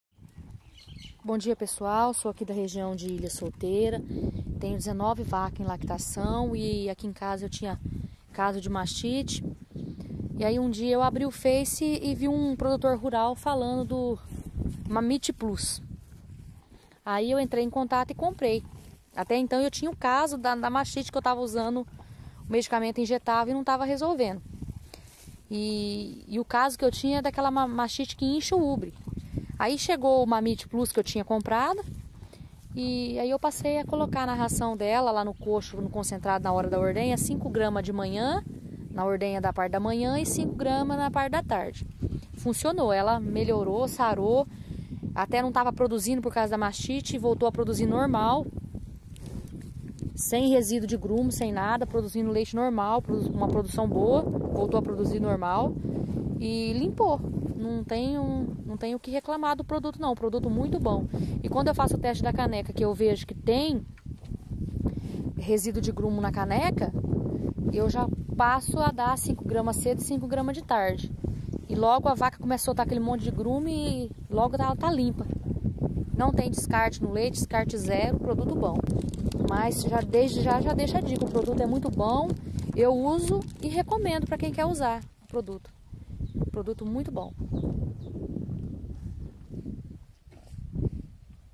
Controle de Mastite com Produto 100% Natural. Depoimento de Cliente.
Cliente, fala sobre CONTROLE DE MASTITE ou MAMITE BOVINA, com o uso do produto MAMITE PLUS

MAMITE PLUS é vendido pela Expressão Animal. Trata-se de produto 100% Natural, Eficaz, sem necessidade de Manejo e Carência Zero, ou seja, sem resíduos: saiba mais ou caso prefira, favor entrar em contato através do

Quer saber mais sobre MAMITE (MASTITE) Bovina?